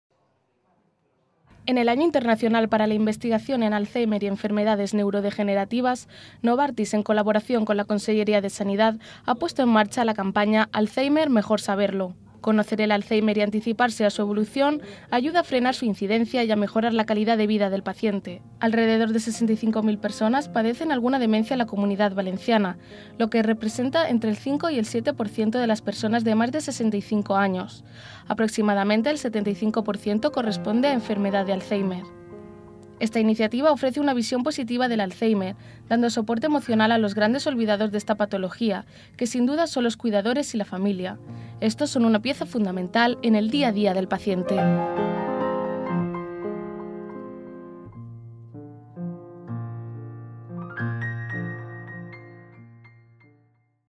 062111 NOT ALZHEIMER.mp4
Conocer el Alzheimer y anticiparse a su evolución ayuda a frenar su incidencia
En el marco del 'Año Internacional para la Investigación en Alzheimer y Enfermedades Neurodegenerativas', el director general de Salud Pública de la conselleria de Sanitat, Manuel Escolano, ha participado hoy en la presentación de la campaña 'Alzheimer, mejor saberlo', una iniciativa de información y concienciación sobre la importancia de la detección del Alzheimer.